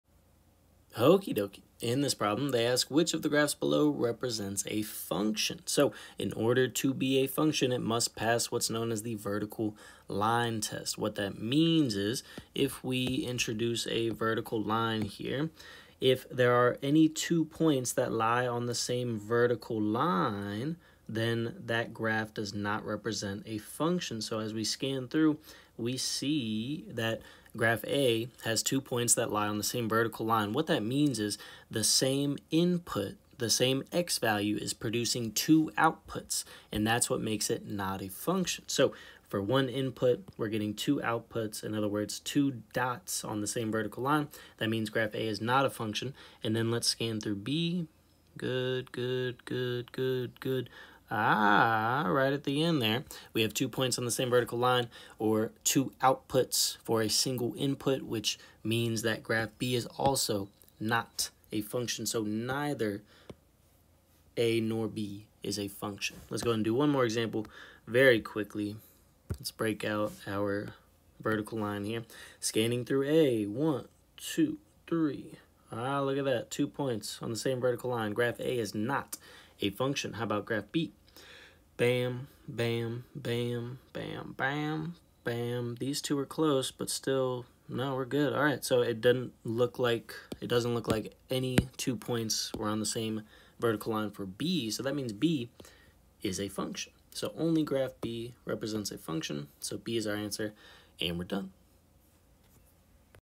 Quiz 2 problem 1.1.3a (VT MATH 1014)
2 examples

Timestamps:
00:00 Example 1
00:05 Vertical Line Test!
01:06 Example 2